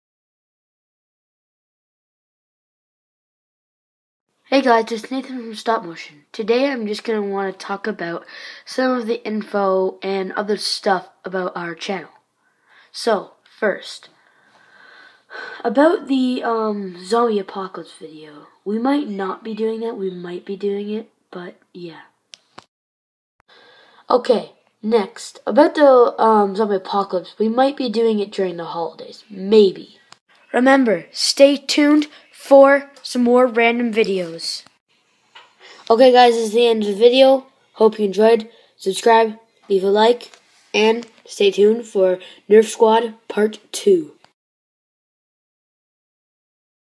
Info
Ok guys in this video I am just telling you guys some stuff about the channel